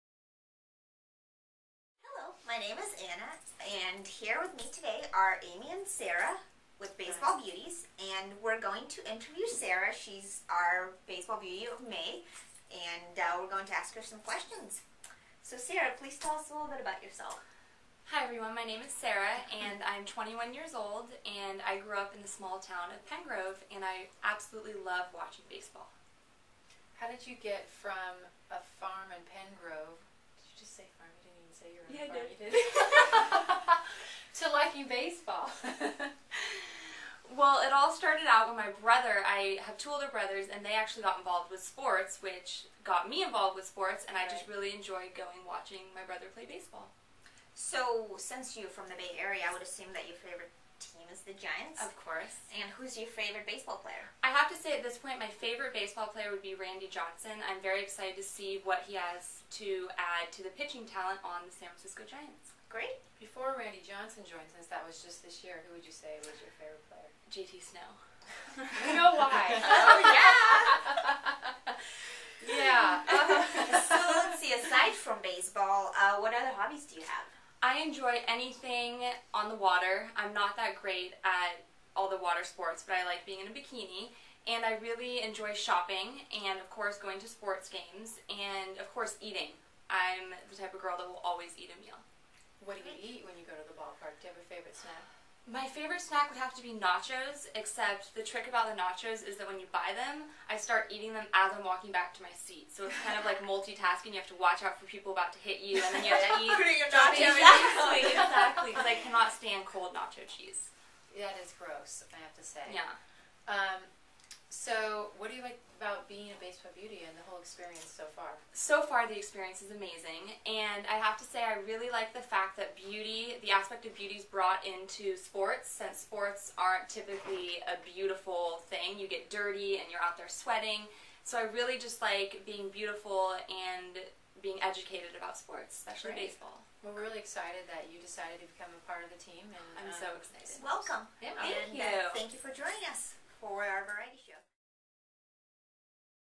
Baseball Beauties Variety Show
Interview with our June Beauty Sarah